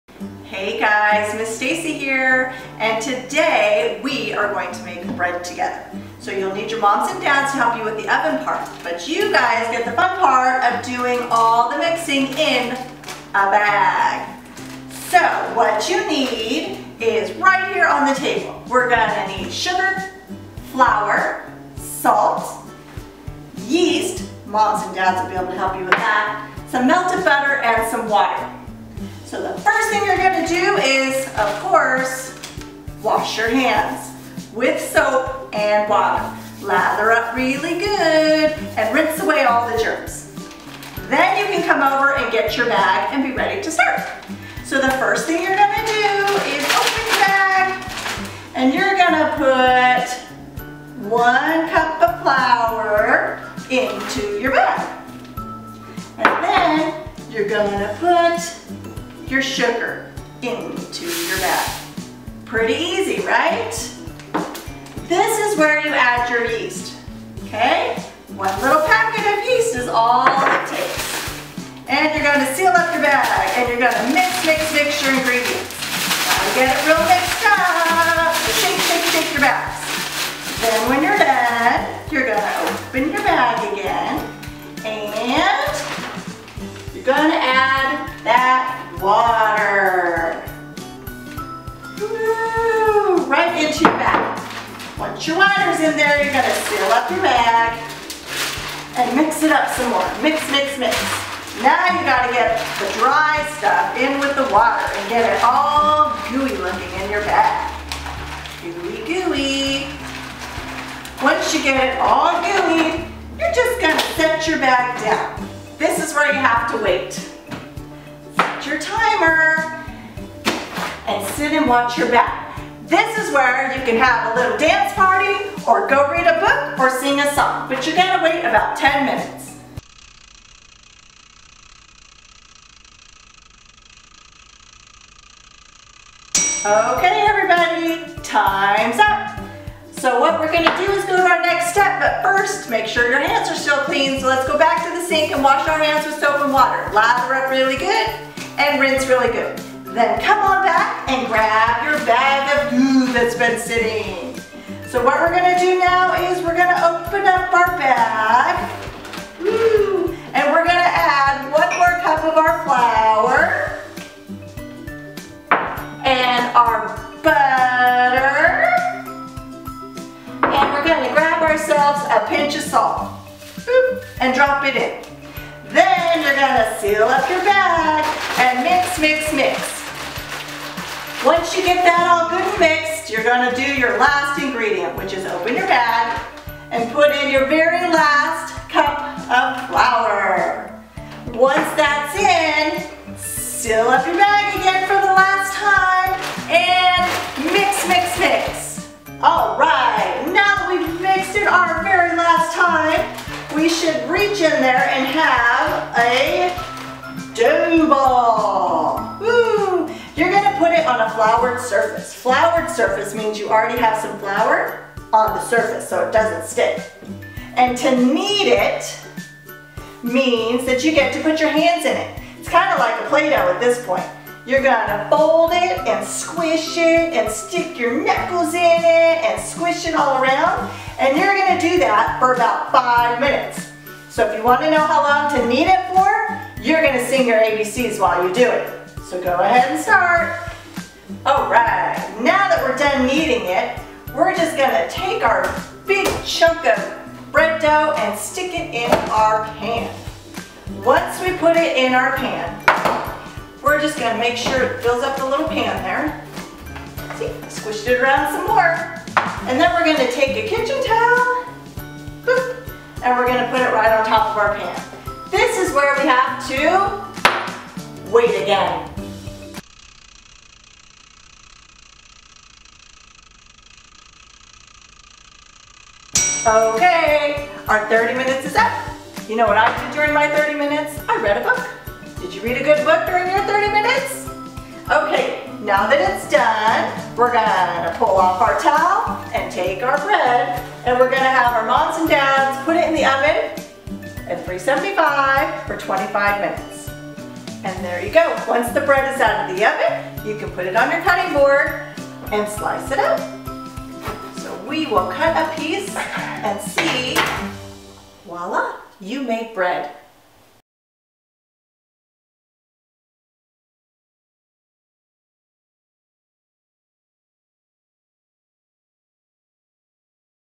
Ms. Staci makes bread!
Enjoy this easy how-to video with Ms. Staci to make delicious bread for everyone!